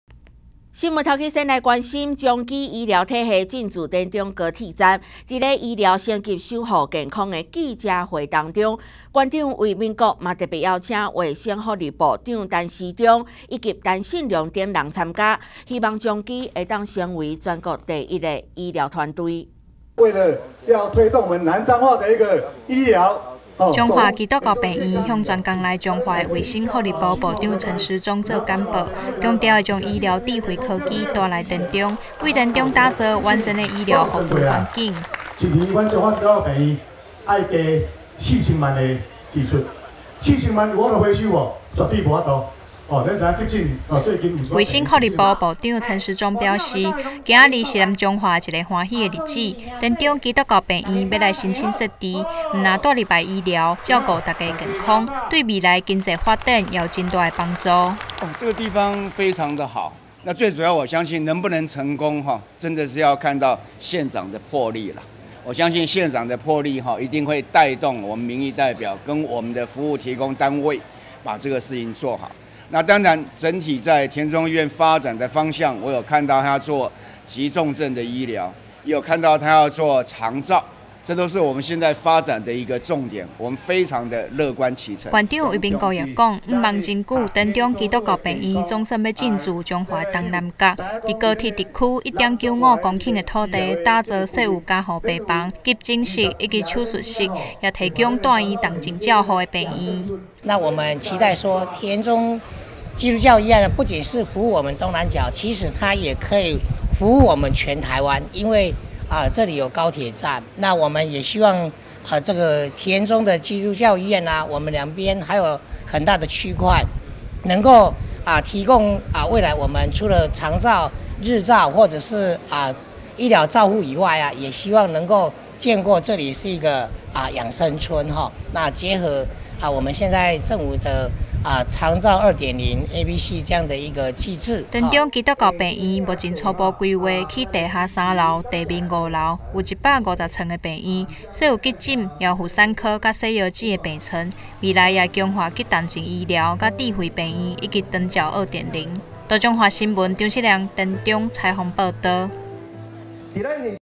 1070413田中基督教醫院進駐高鐵站區 提供日照及醫療照護
充實南彰化醫療資源，彰化基督教醫院將在田中高鐵站區提供在地化的醫療服務，衛生福利部長陳時中特地南下聽取簡報，並肯定未來的田中基督教醫院提供南彰化地區包括急重症醫療、婦產科等醫療照護，更配合長照2.0提供日照及醫療照護！